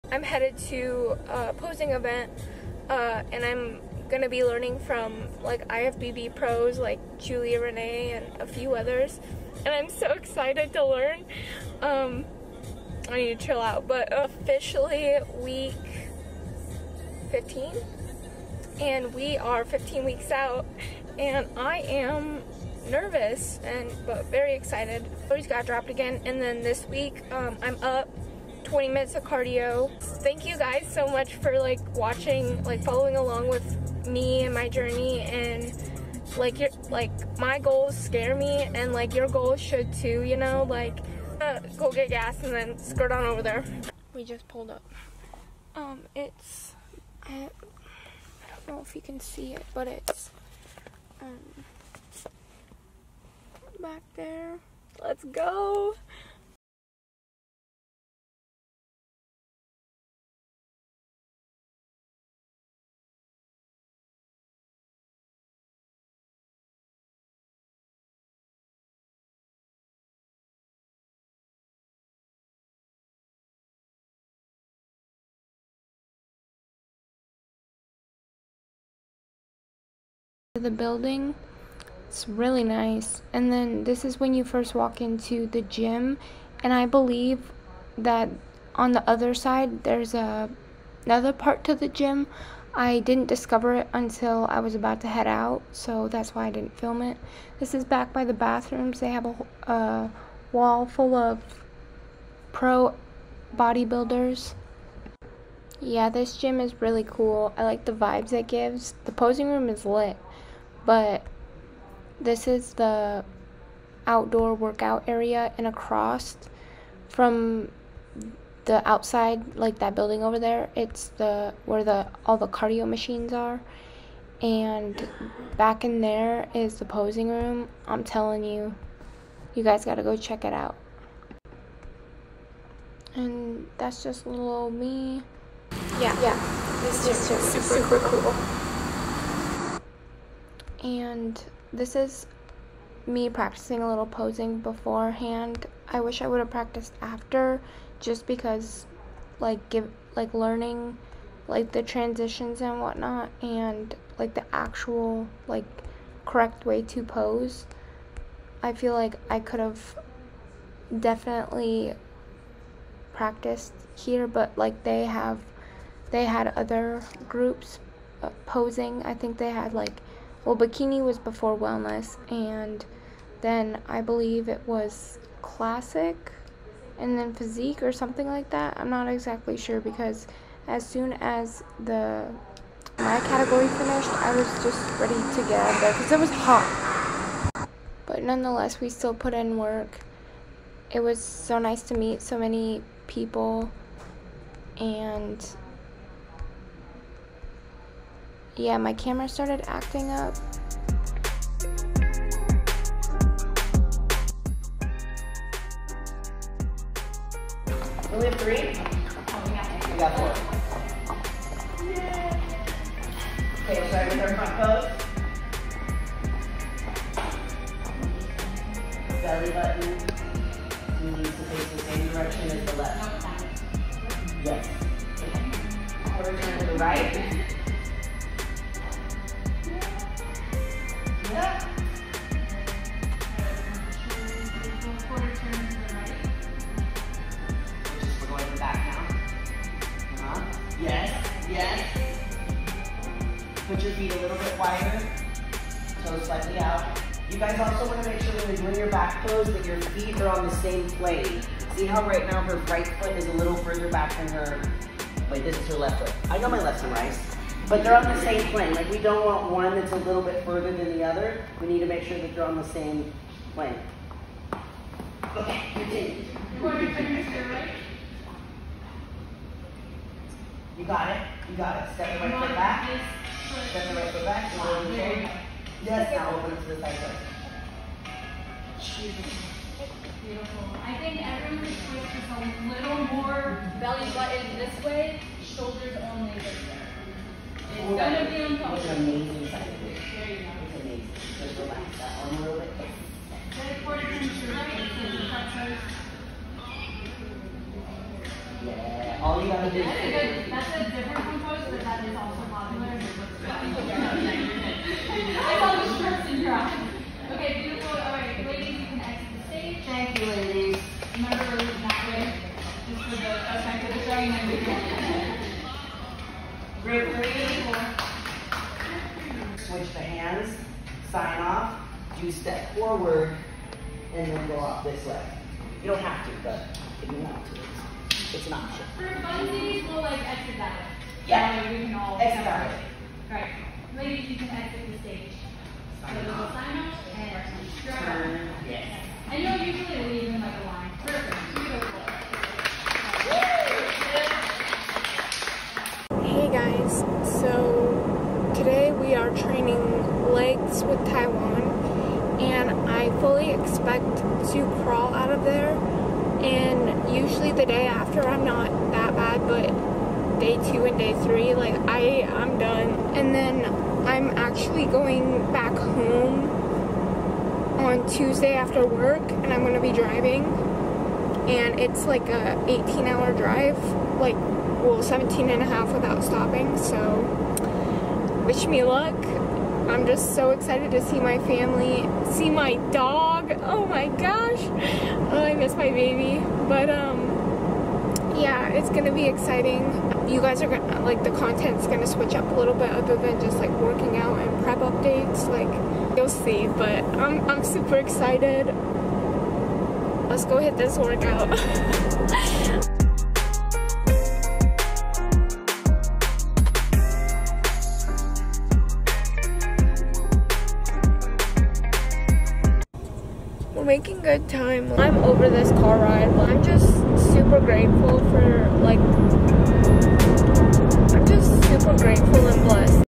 Iron City | IFBB Pro Wellness Pros teach posing @ HTX Posetown | Road-trip across the country
Big thank you to IFBB pros Julia Rene and Kendall Aliyah for teaching me how to properly pose and transition on stage. Thank you to Iron Coty for putting on this event.

Week 16 into prep and it has been a challenge, but we continue to push.

Extremely blessed and grateful for the community and friends I have made along this journey. For everything to be completely honest, truly grateful 🥹 🙏 Thank you